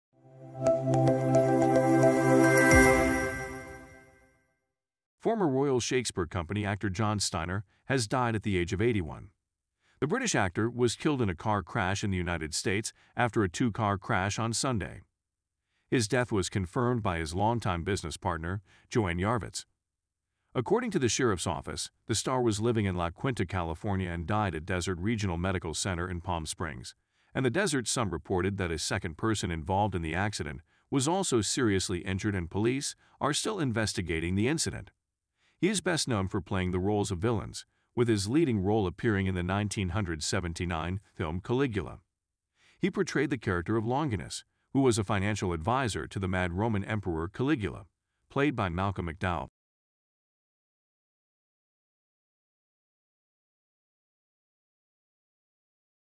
Caligula actor John Steiner killed in stateside car crash
Former Royal Shakespeare Company actor John Steiner has died at the age of 81.

The British actor was killed in a car crash in the United States after a two-car crash on Sunday.

His death was confirmed by his longtime business partner, Joanne Yarvitz.

According to the sheriff's office, the star was living in La Quinta, California and died at Desert Regional Medical Center in Palm Springs, and the Desert Sun reported that a second person involved in the accident was also seriously injured and police are still investigating the incident. .

He is best known for playing the roles of villains, with his leading role appearing in the 1979 film Caligula.

He portrayed the character of Longinus, who was a financial advisor to the mad Roman Emperor Caligula, played by Malcolm McDowell.